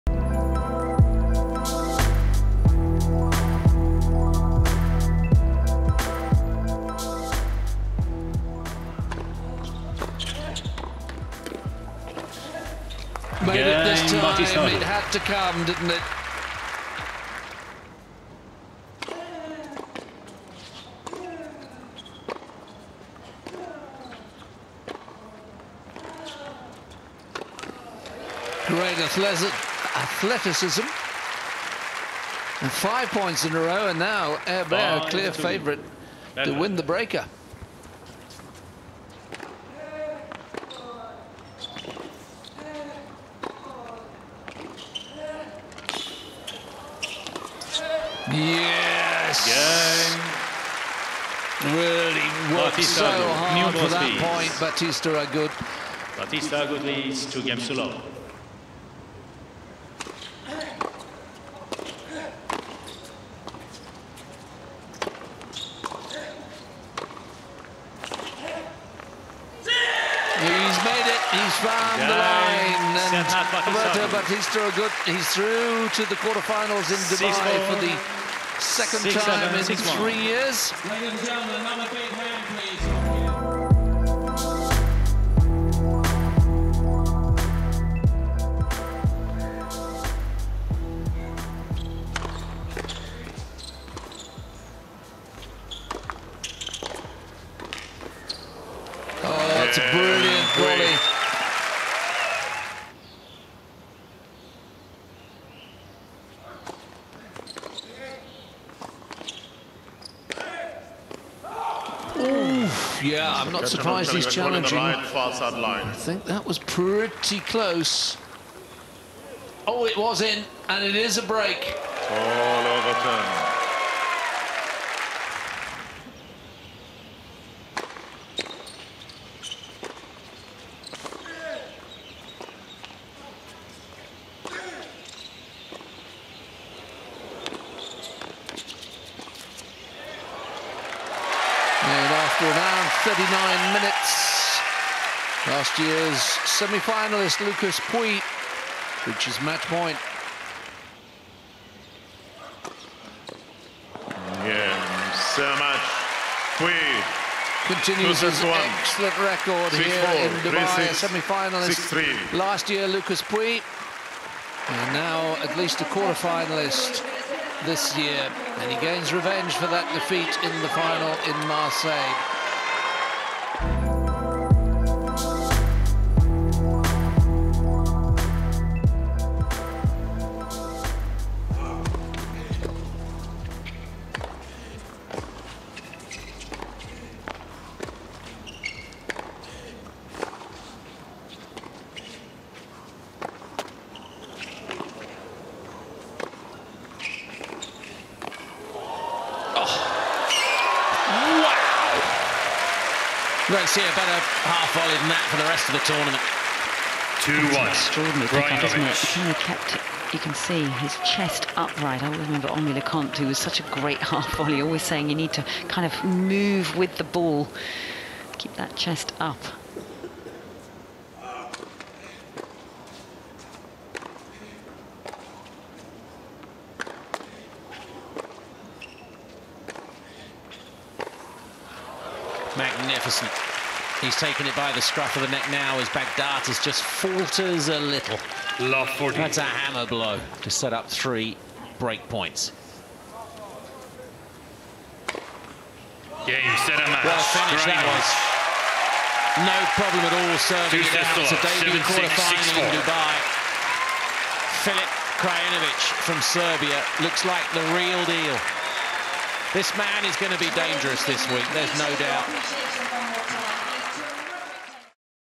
R16 Highlights: Bautista Agut, Pouille, Krajinovic Into QFs - Dubai Tennis 2018
No. 2 seed Lucas Pouille and No. 3 seed Roberto Bautista Agut were among those booking quarter-final berths at the 2018 Dubai Duty Free Tennis Championships.